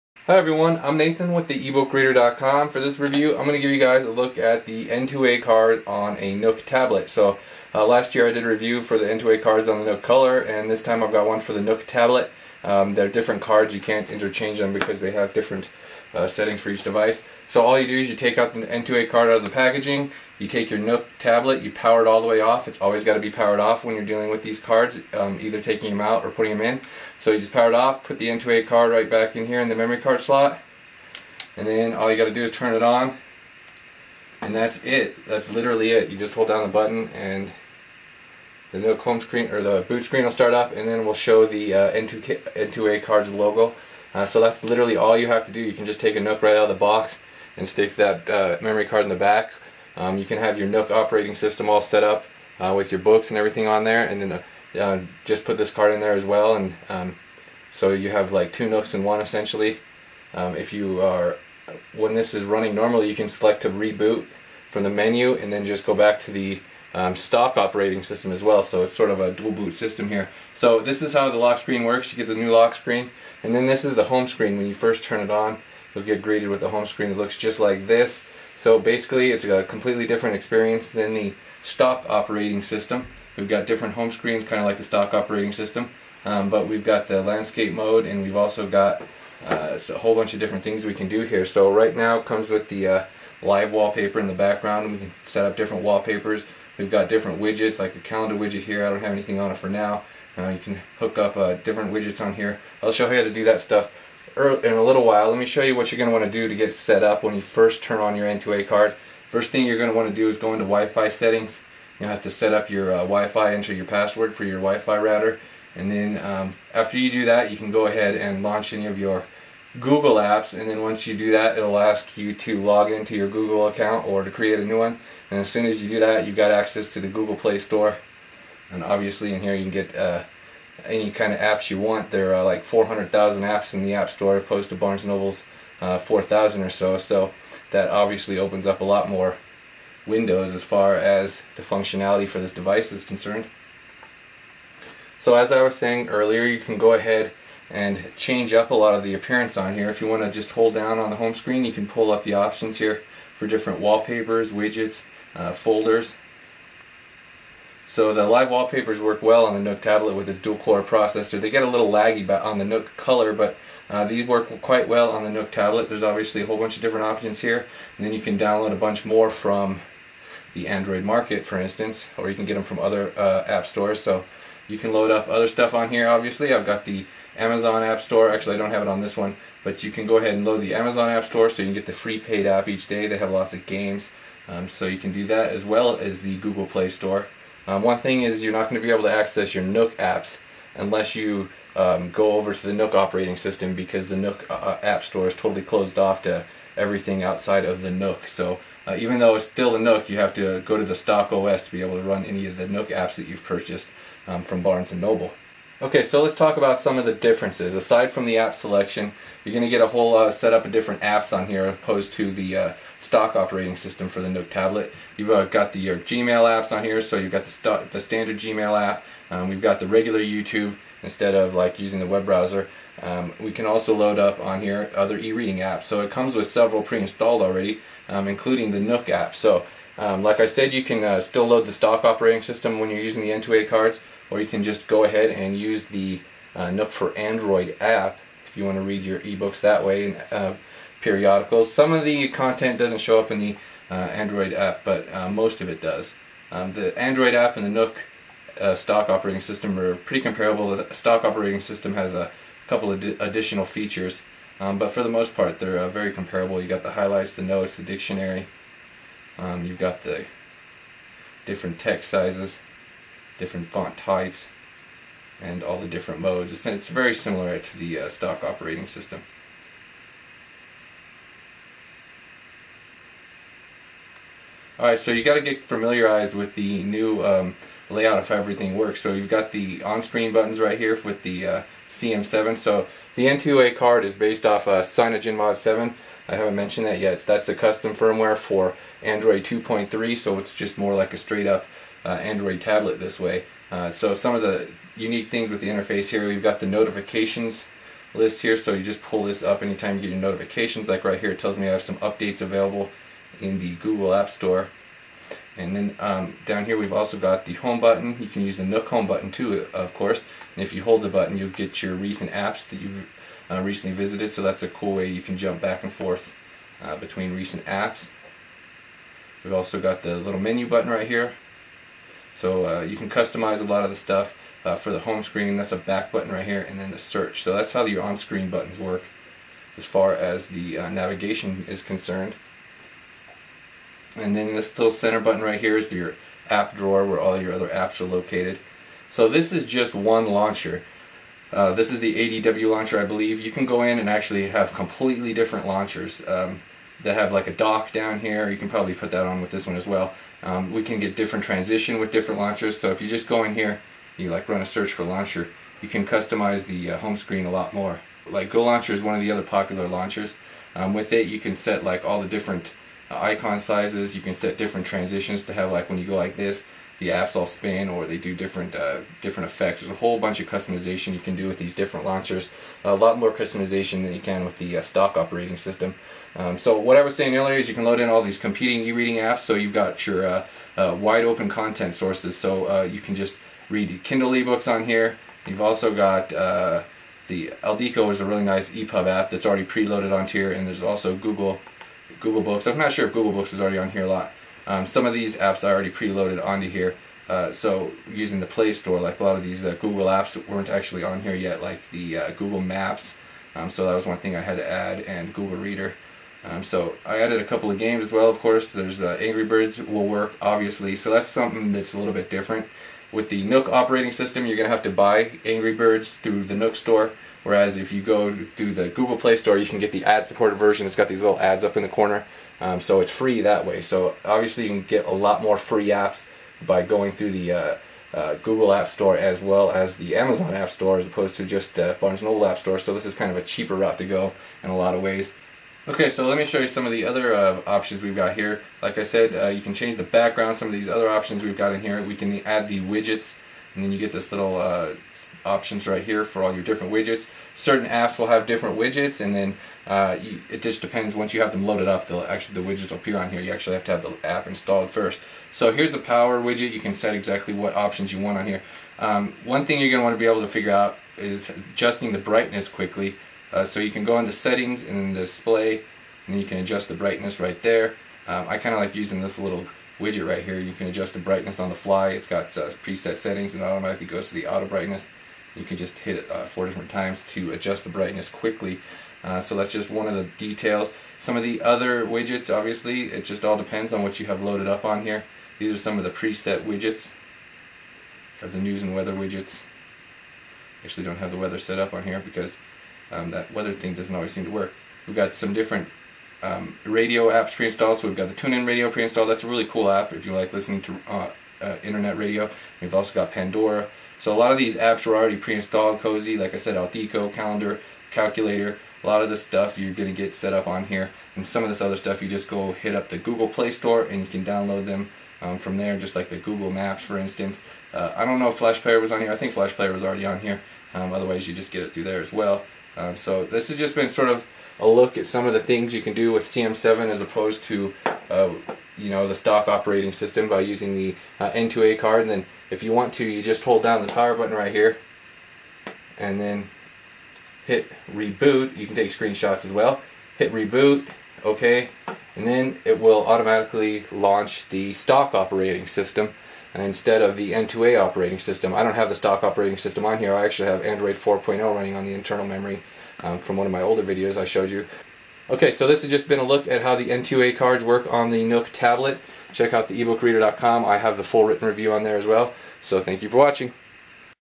Review: N2A Cards for Nook Tablet - Plus Tips and Tricks for Getting Started
Written Review This video is a look at the N2A cards for the Nook Tablet for CyanogenMod 7, a custom aftermarket firmware based on Android 2.3 Gingerbread. Here's the link for N2A cards at Amazon